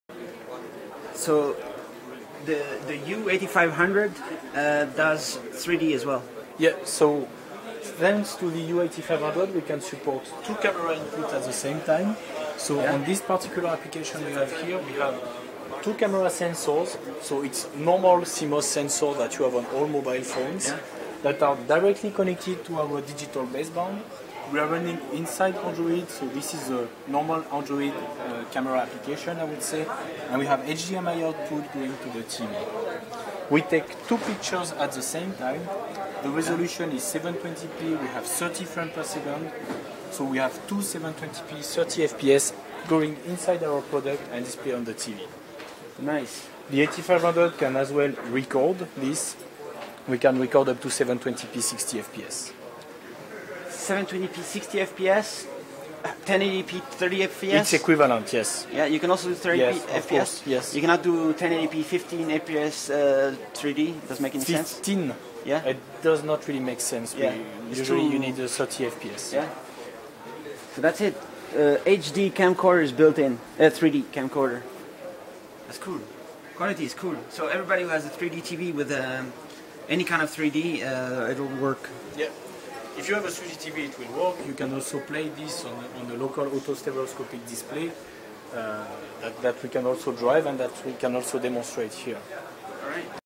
3D capture and playback on the ST Ericsson U8500 platform
ST Ericsson U8500 can capture 720p 3D at 30fps and 1080p 2D at 30fps. Here's a demo of the 3D effect captured using 2 cameras and displayed on a 3D HDTV.